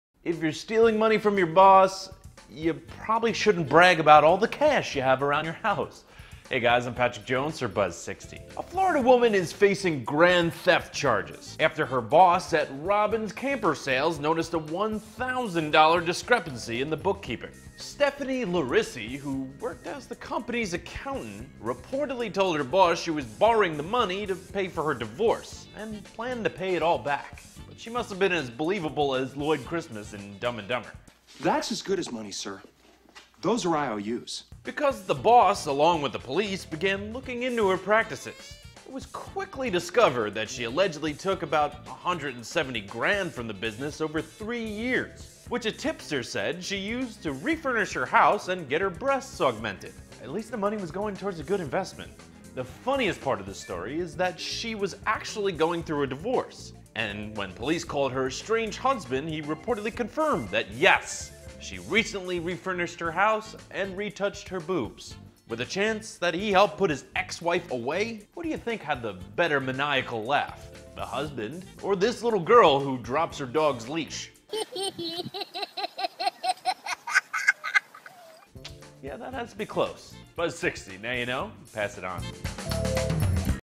BOOB CROOK Woman Arrested For Allegedly Stealing $170K For Boob Job
Girl got to do what a girl Got to do  BOOBS CROOK   Florida Woman Arrested For Allegedly Stealing $170K For Boob Job